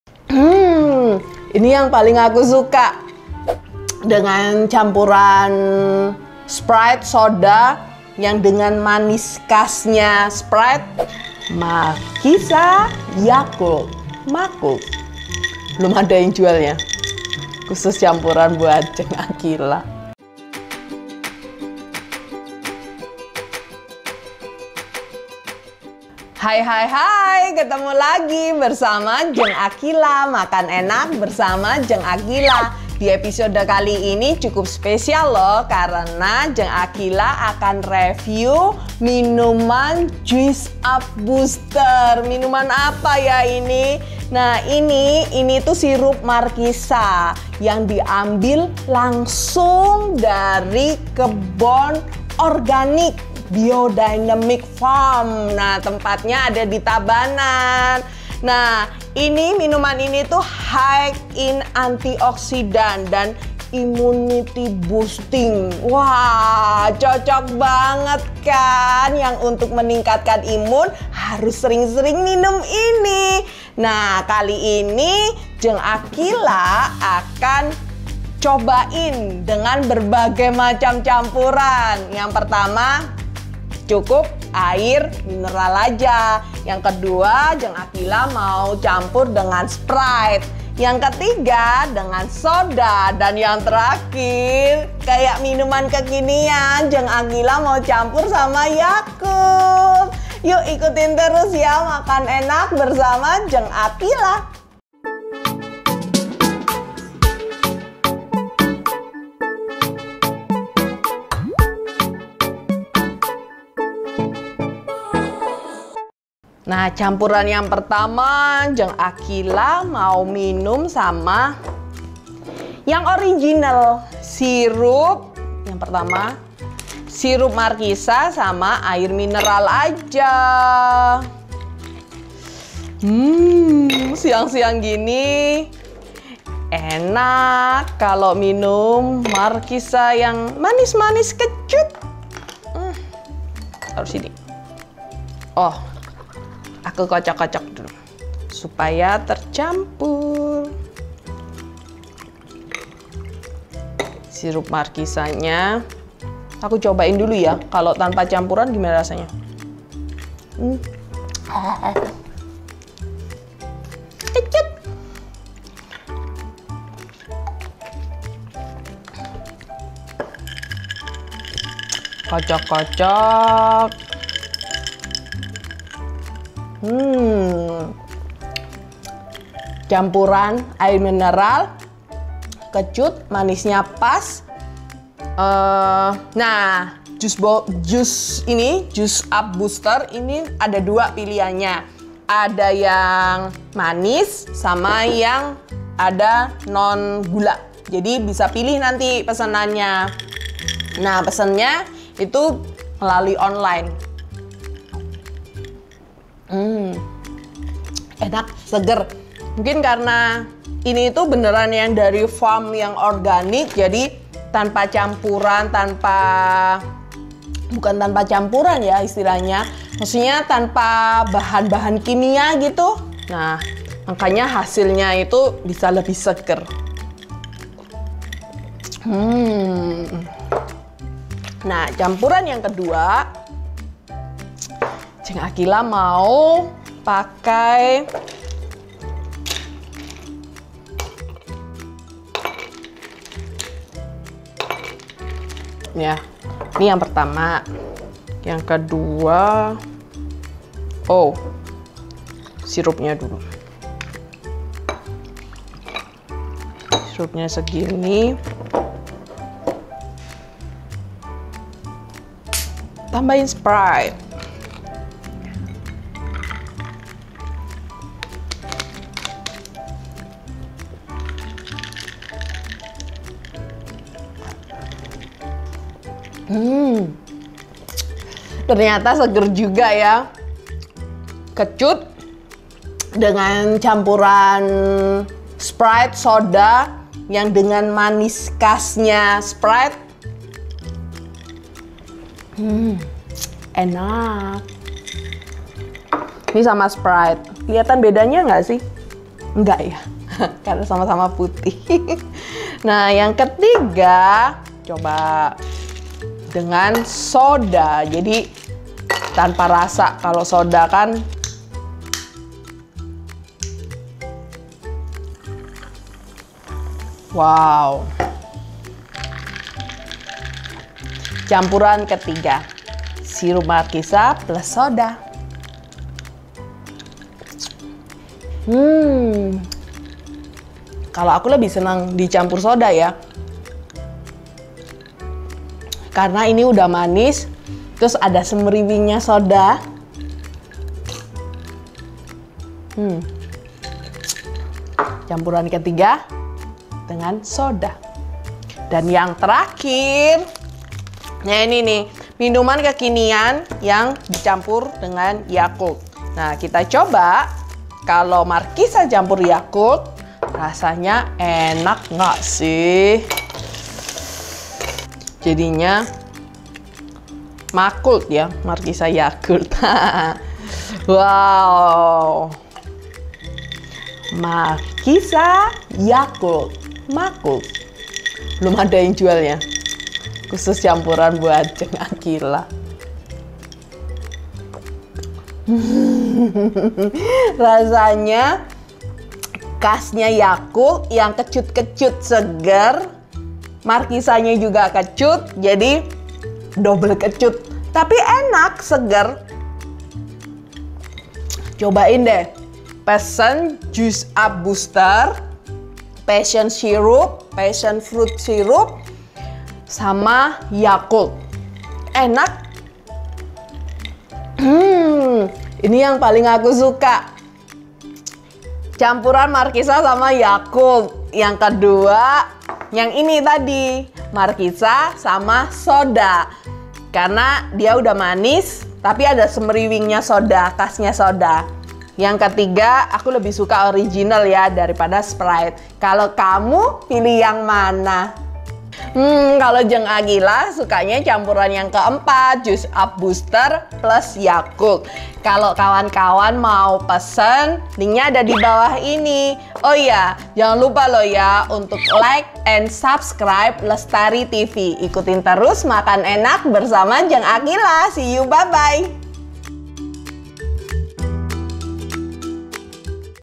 Mukbang With Jeng Aqila Episode 10 - Juice Up Booster, Passion Fruit to Boost your Health
Tak terasa, 10 episode sudah Jeng Aqila menemani viewer Lestari TV, sekaligus video ini, menjadi episode final dari program Mukbang With Jeng Aqila. Dan karena penutup, maka Jeng Aqila pun mengulas produk yang tak biasa, yakni Juice markisa berlabel Juice Up Booster.

Produk ini hasil olahan UKM di Bali yang bahan bakunya dari kebun sendiri yang organik. Menariknya, Jeng Aqila mengombinasikan juice markisa dengan campuran yang berbeda-beda, mulai dari air mineral, sprite sampai dengan Yakult.

Seperti apa sensasi rasanya, dan apa campuran favorit dari Jeng Aqila?

Simak lengkap dalam video ini ya, dan pastikan teman-teman pun bisa merasakan sensasi yang sama.

Sampai jumpa di program yang lain, dari Jeng Aqila, See you bye bye..!